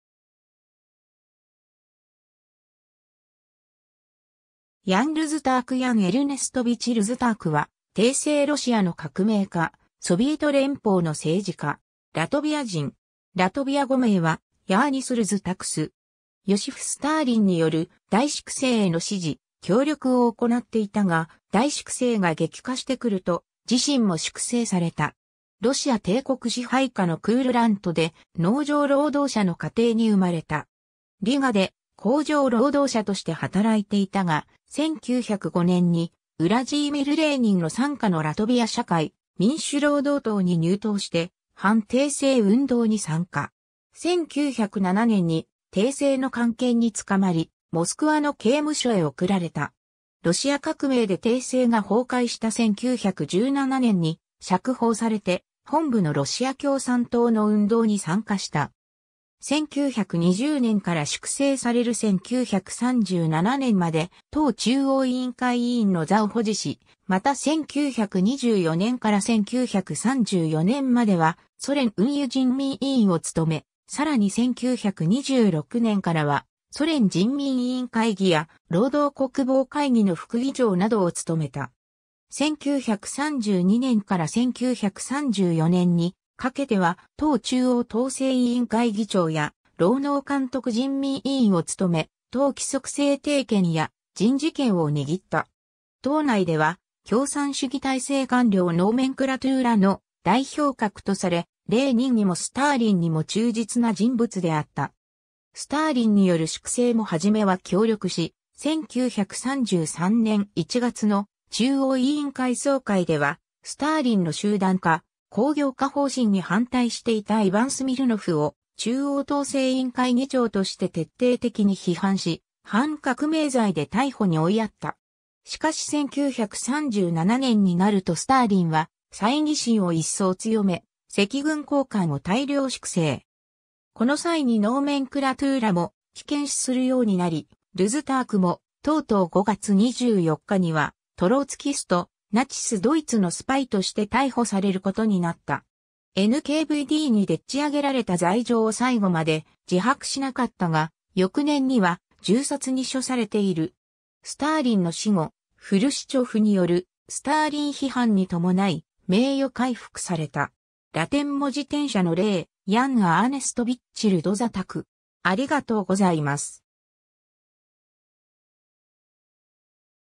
ヤン・ルズターク
全連邦共産党（ボリシェヴィキ）中央委員会組織局の人物
ロシア・ソビエト連邦社会主義共和国の政治家
ヤン・ルズターク ヤン・エルネストヴィチ・ルズターク（ロシア語: Ян Эрнестович Рудзутак, 1887年8月3日 - 1938年6月29日 ）は、帝政ロシアの革命家、ソビエト連邦の政治家。
ラトビア人。
ラトビア語名はヤーニス・ルズタクス（Jānis Rudzutaks）。
 ヨシフ・スターリンによる大粛清への支持・協力をおこなっていたが、大粛清が激化してくると自身も粛清された。
 ロシア帝国支配下のクールラント（現：ラトビア）で農場労働者の家庭に生まれた。
リガで工場労働者として働いていたが、1905年にウラジーミル・レーニンの傘下のラトビア社会民主労働党に入党して反帝政運動に参加。
1907年に帝政の官憲に捕まり、モスクワの刑務所へ送られた。
ロシア革命で帝政が崩壊した1917年に釈放されて本部のロシア共産党の運動に参加した。
 1920年から粛清される1937年まで党中央委員会委員の座を保持し、また1924年から1934年まではソ連運輸人民委員（運輸相）をつとめ、さらに1926年からは、ソ連人民委員会議や労働国防会議の副議長などをつとめた。
1932年から1934年にかけては党中央統制委員会議長や労農監督人民委員をつとめ、党規則制定権や人事権を握った。
党内では共産主義体制官僚ノーメンクラトゥーラの代表格とされ、レーニンにもスターリンにも忠実な人物であった。
 スターリンによる粛清もはじめは協力し、1933年1月の中央委員会総会ではスターリンの集団化・工業化方針に反対していたイヴァン・スミルノフ（通信人民委員）を中央統制委員会議長として徹底的に批判し、反革命罪で逮捕に追いやった。
 しかし1937年になるとスターリンは猜疑心を一層強め、赤軍高官を大量粛清。
この際にノーメンクラトゥーラも危険視するようになり、ルズタークもとうとう5月24日には「トロツキスト」「ナチス・ドイツのスパイ」として逮捕されることになった。
NKVDにでっち上げられた罪状を最後まで自白しなかったが、翌年には銃殺に処されている。
スターリンの死後、フルシチョフによるスターリン批判に伴い、名誉回復された。
 ^ ラテン文字転写の例: Yan Ernestovich Rudzutak